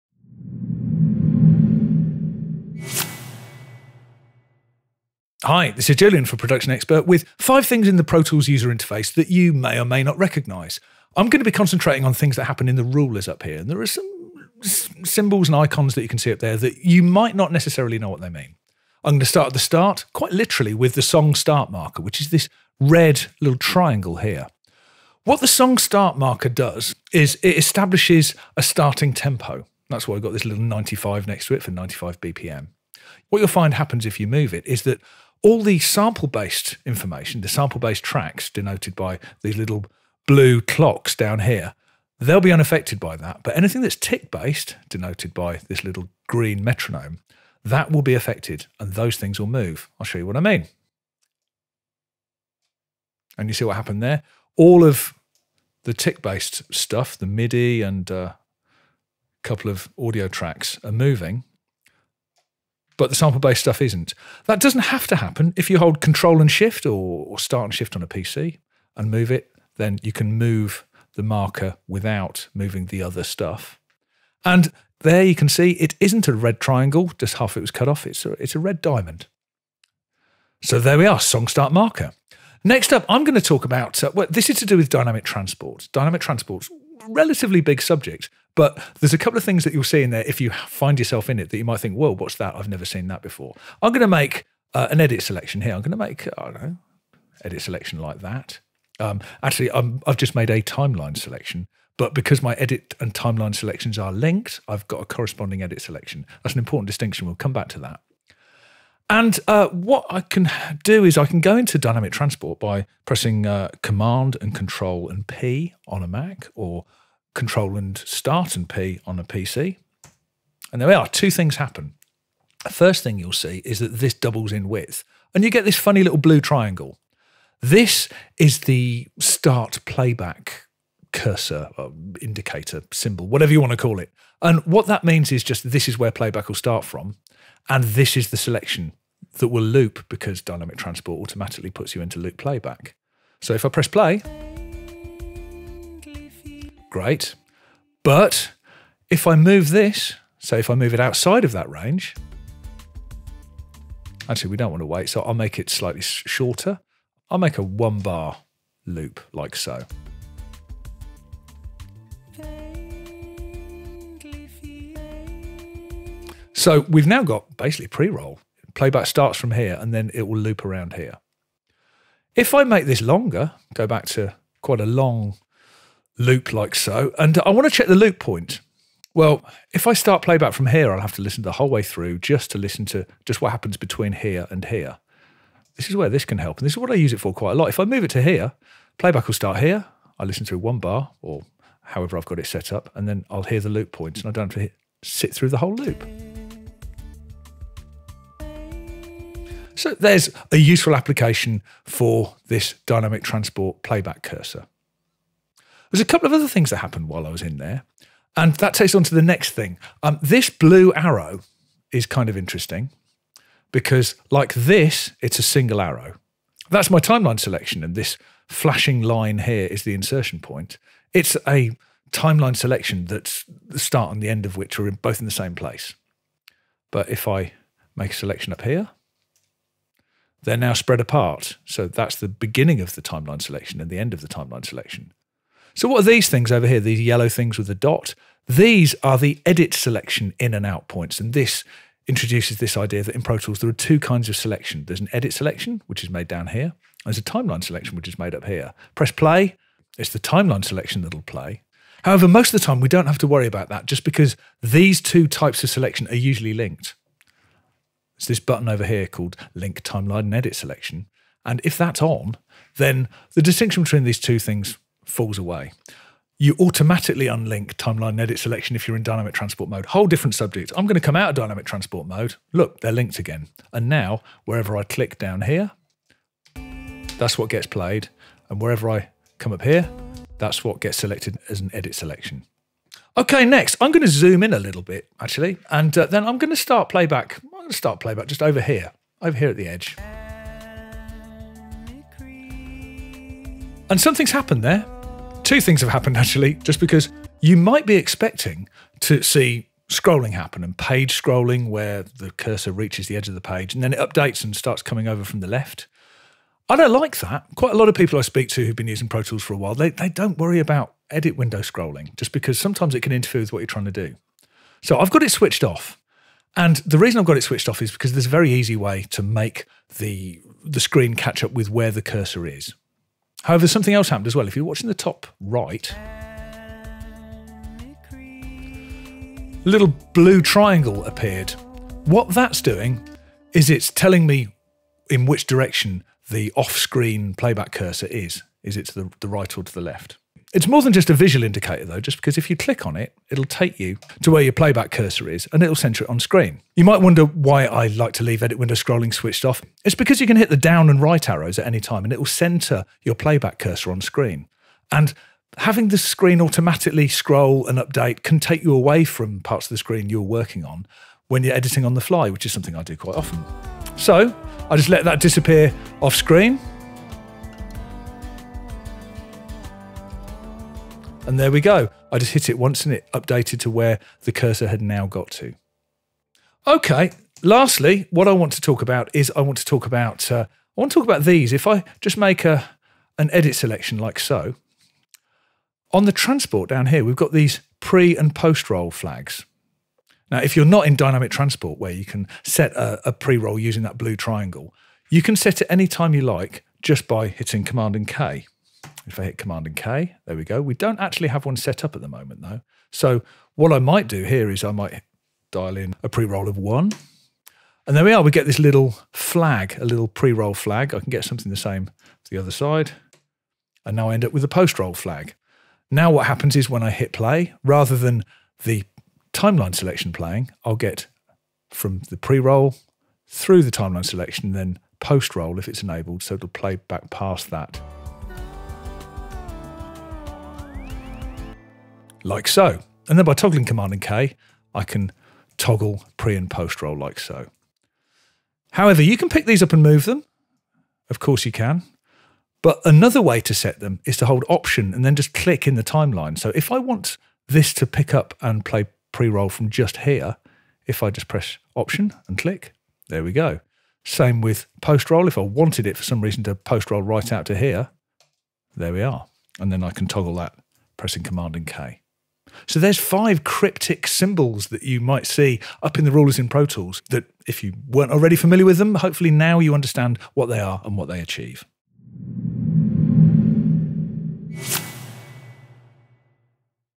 5 Symbols In The Pro Tools Rulers And What They Mean
In this video Julian explains what some of the symbols the rulers in Pro Tools display mean and how they are useful.